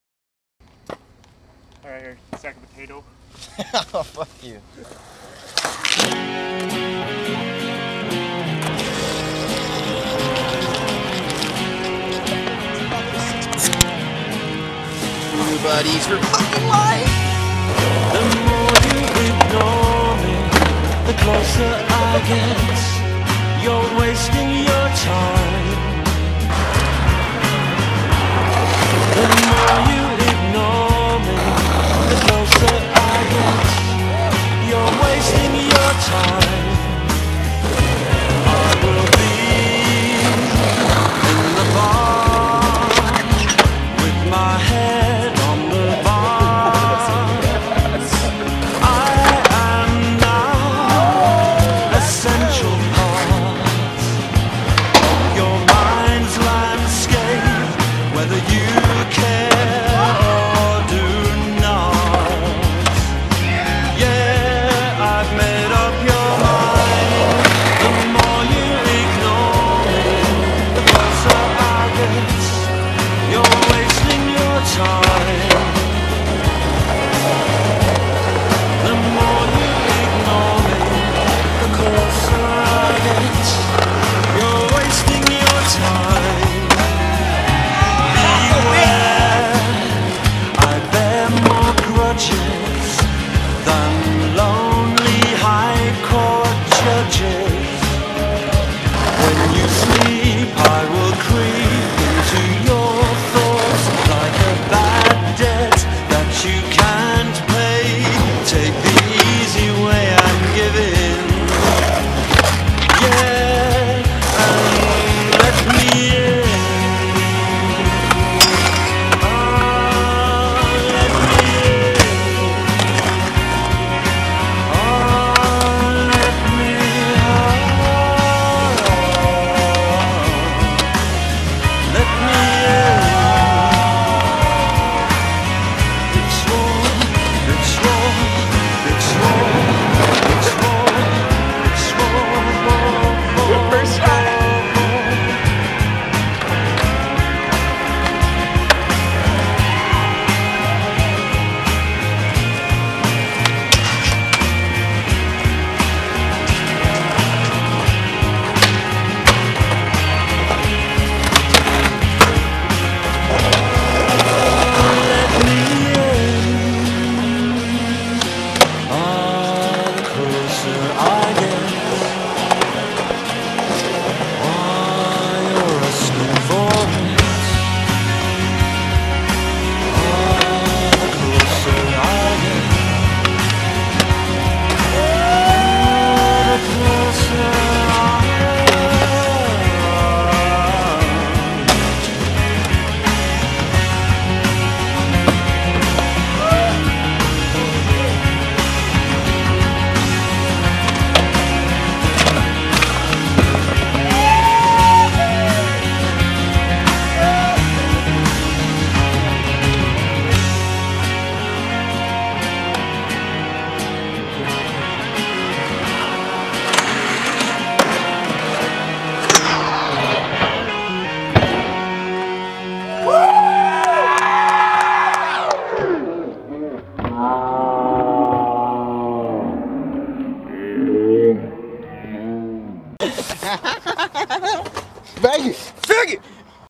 Max Fisher Footage Party 2 RE EDIT
Jaxster never liked his song in pt. 2 truth be told i never really did either... heres a re edit with some extra footy too, enjoy!  pt. 3 soon?

filmed by; Mike Bricke, Bradd Salazar, Bronson Ayub, Jet Caputo, Vin Dog, and more.